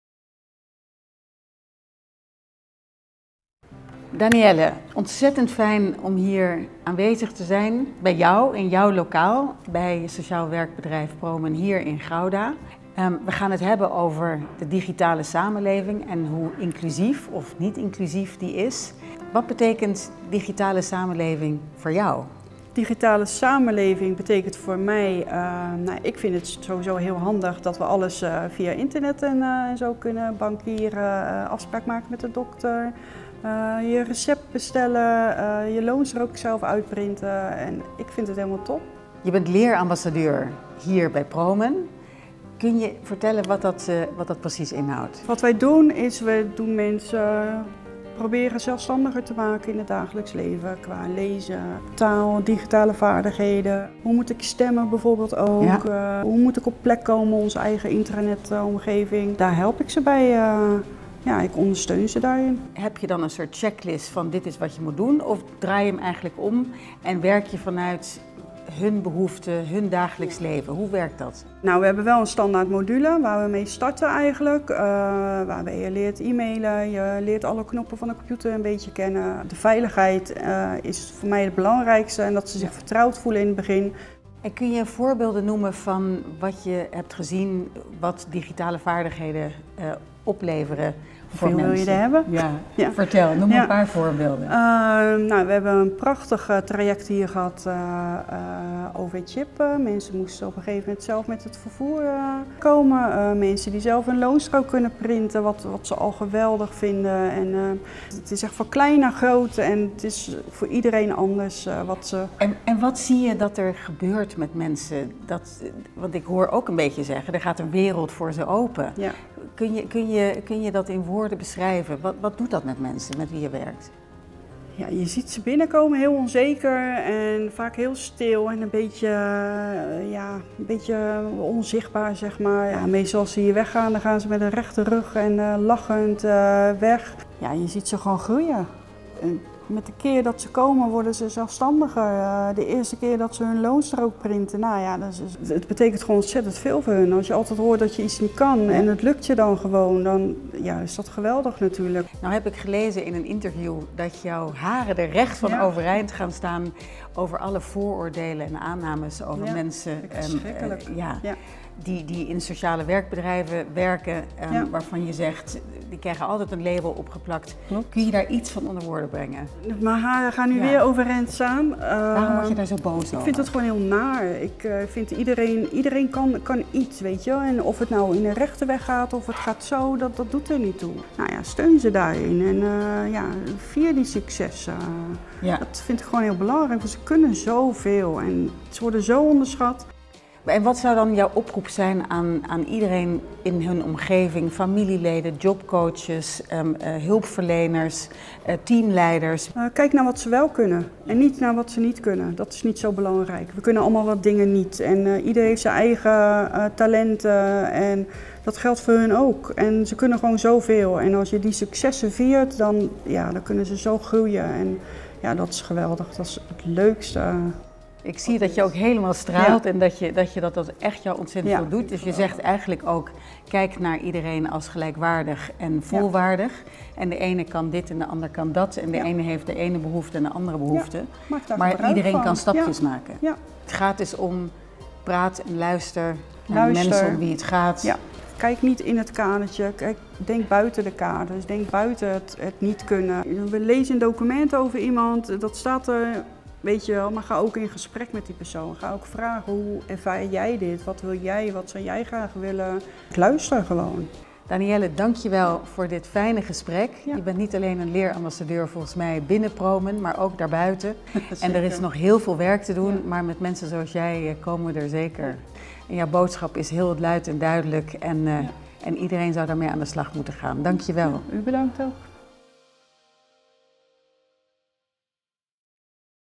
Prinses Laurentien in gesprek over digitale vaardigheden
Prinses Laurentien op bezoek bij Promen. Een medewerker vertelt hoe zij haar collega’s helpt bij het digitaalvaardiger worden.